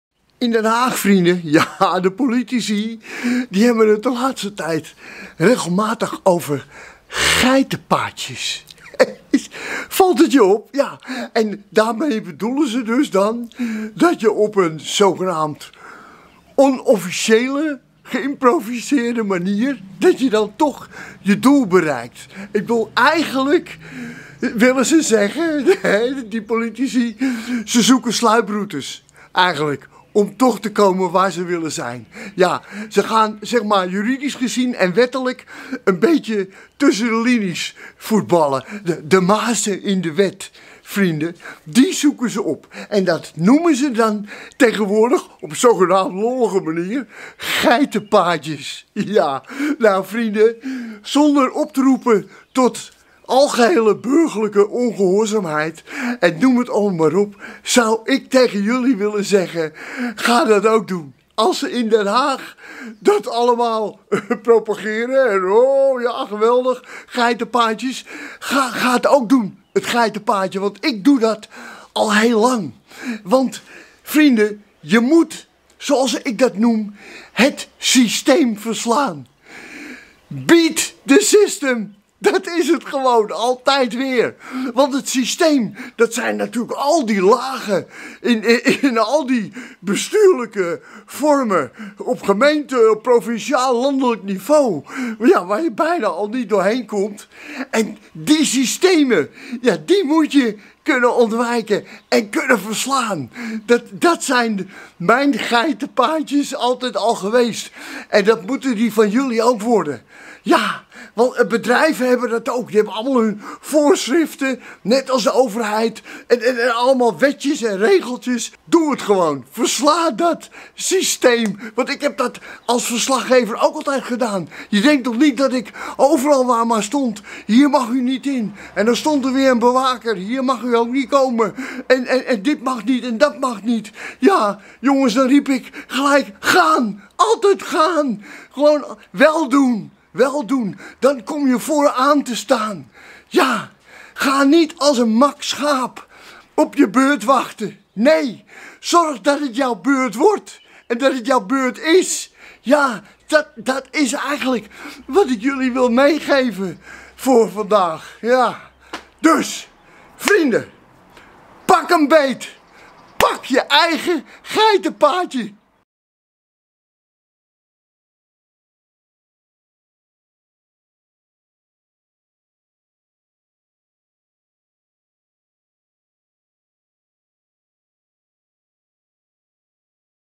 Versla het systeem, pak een geitenpaadje!
In Den Haag hebben ze het veel over 'geitenpaadjes'. Vrienden het wordt de hoogste tijd dat jullie dat ook gaan doen. Versla het systeem!